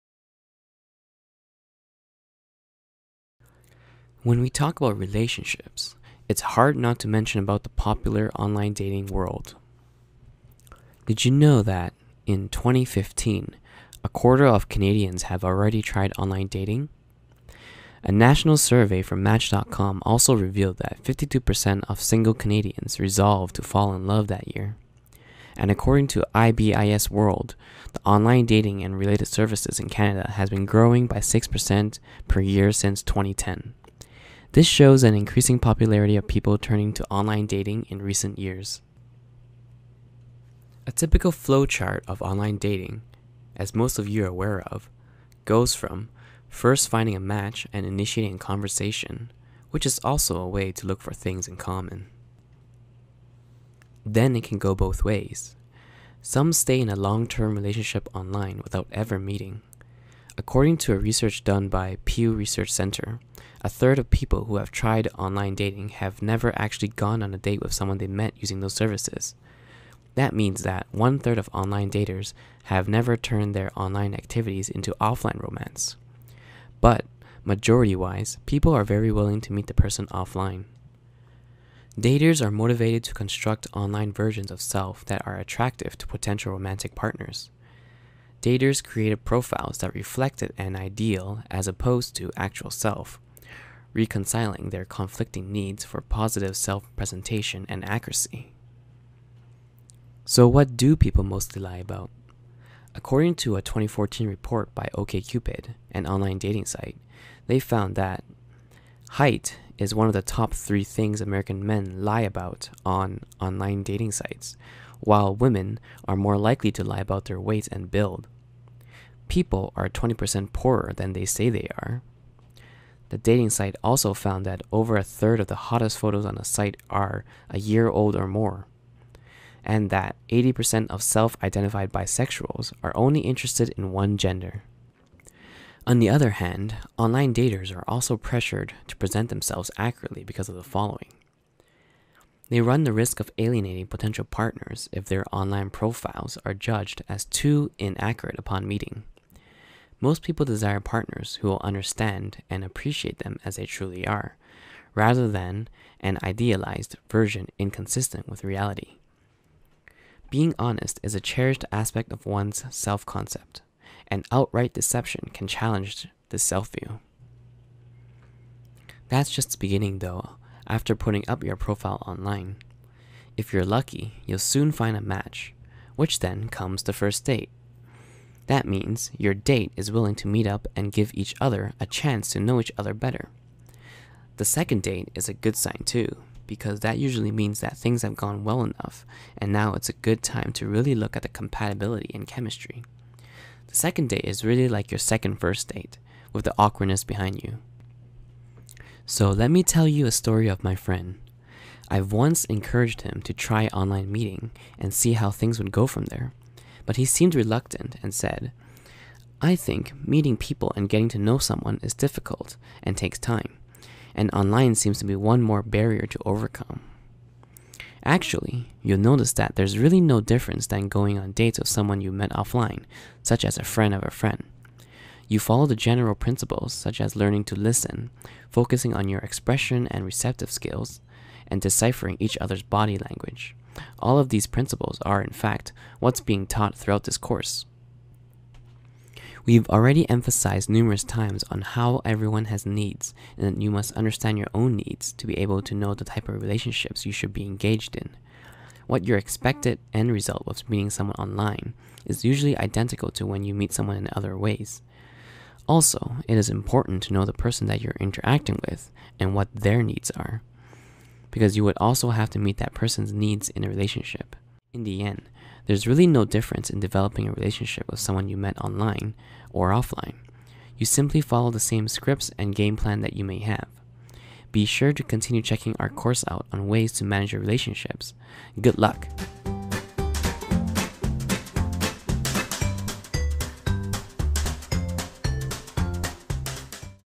Online Dating - The Arts and Science of Relationships: Understanding Human Needs
This course provides an introduction to:
1. Basic concepts of The Strategies and Skills Learning and Development System (SSLD), their relevance for every day relationships and provide advanced concepts for  participants who work in fields of social work and health care .
2. Basic practice principles and methods of SSLD, illustrated by relationship management case studies.
3. The SSLD framework for relationship management assessment; N3C (needs, circumstances, characteristics, capacity) and problem translation.
4. Core competencies in the relationship management application of the SSLD system: Observation learning, simulation, real life implementation, review and monitoring.
Psychology, Assertiveness, Communication, Building Relationships
The course contents are somewhat specialised and I'm glad it's offered free of charge.  Learning this course helps in understanding dysfunctional relationship and how to change it from yourself,It'a an excellent and a very interesting course not just for people trying to make a career as a social worker but for anyone pursuing any field to help learn learn navigate relationships better